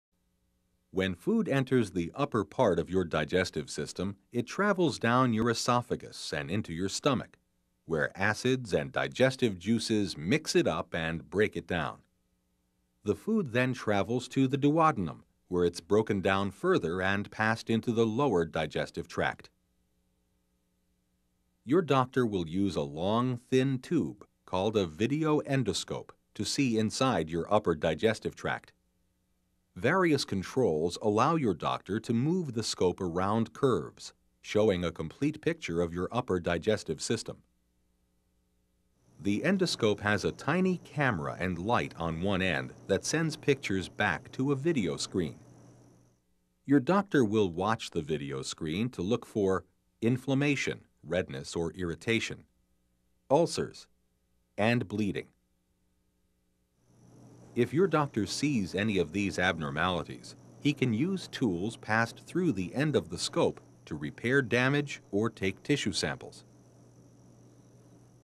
Tutorial 1 Page 3 - Upper GI Endoscopy
This is a brief demonstration of upper GI endoscopy for education purpose in the stimulated PBL session of Surgical Taster Day 2016.

Student Surgical Association of Hong Kong